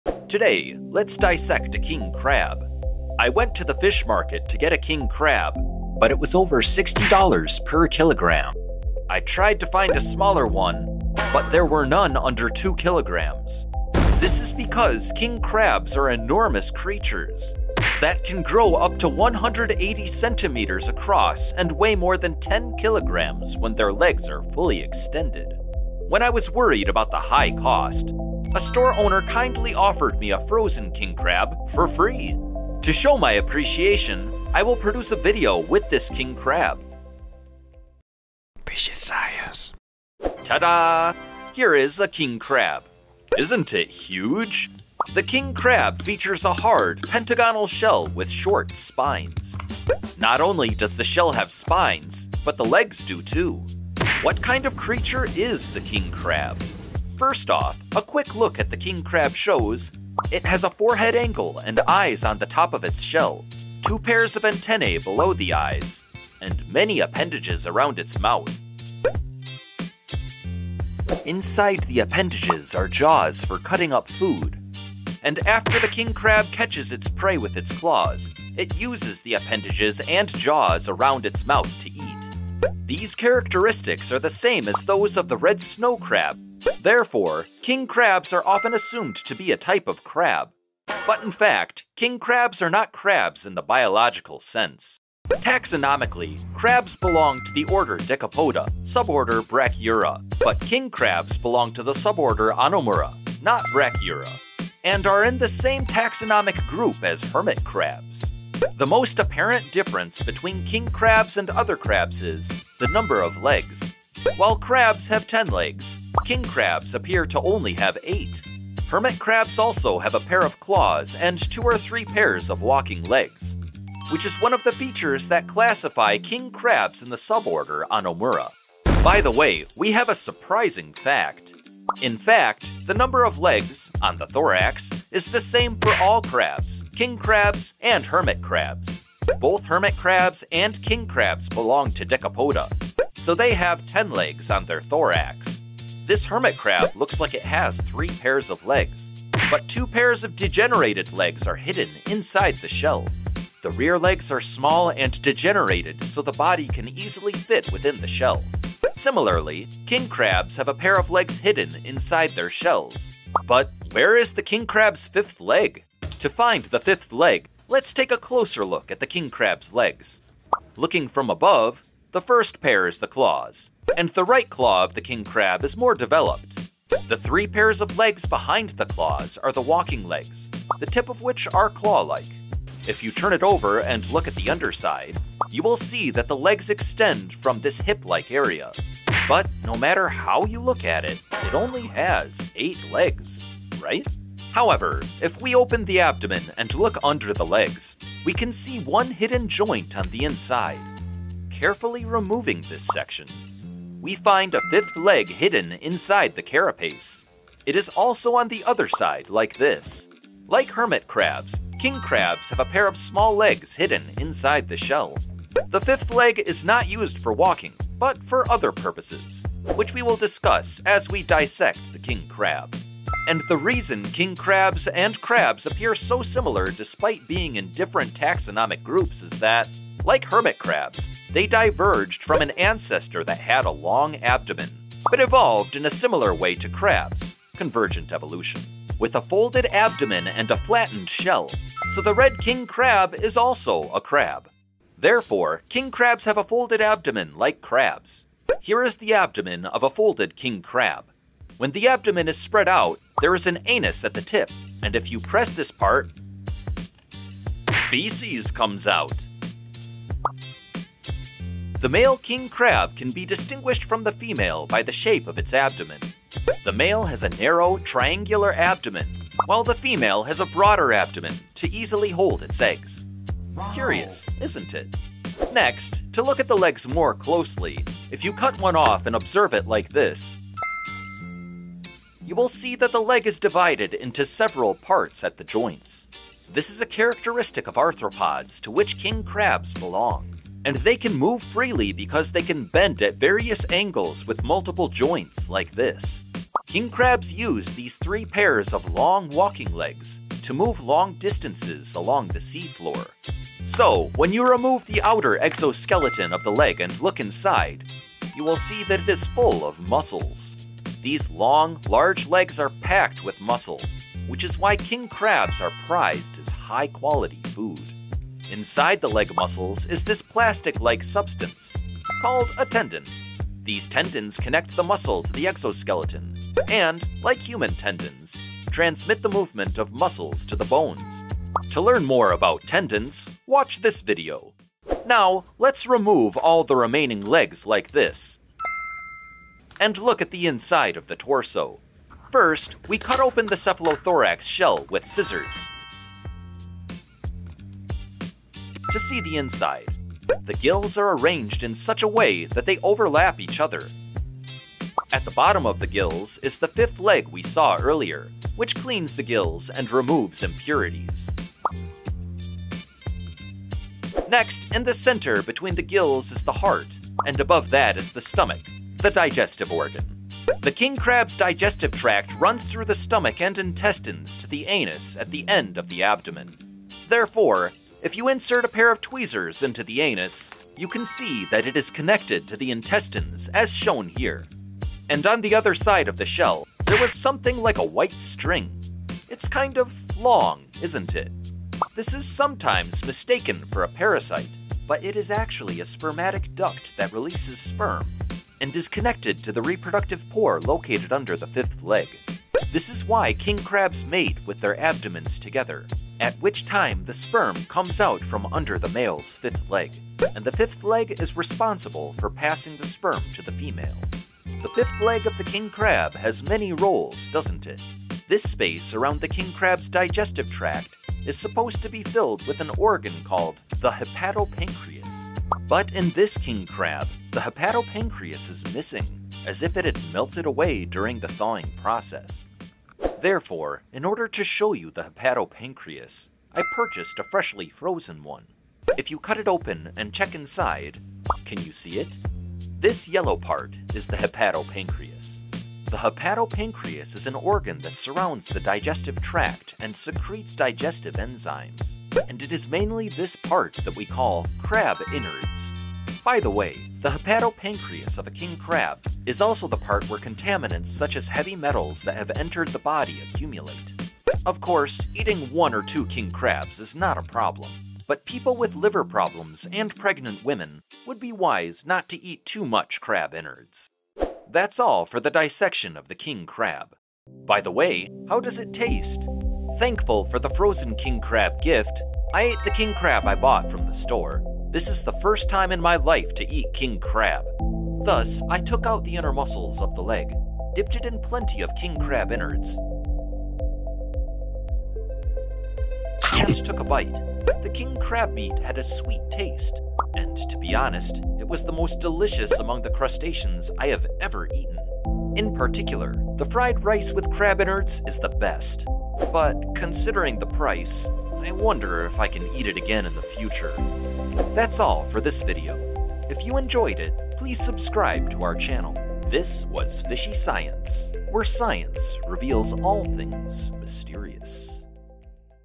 Why the King Crab is Not a "Crab" - King Crab Dissection
Welcome to the intriguing world of the king crab! Often mistaken for having eight legs, king crabs actually share more in common with their crustacean cousins than meets the eye. But if it's not the number of legs, what really sets the king crab apart from other crabs? Dive into this video to uncover the hidden differences and learn why king crabs are such fascinating creatures.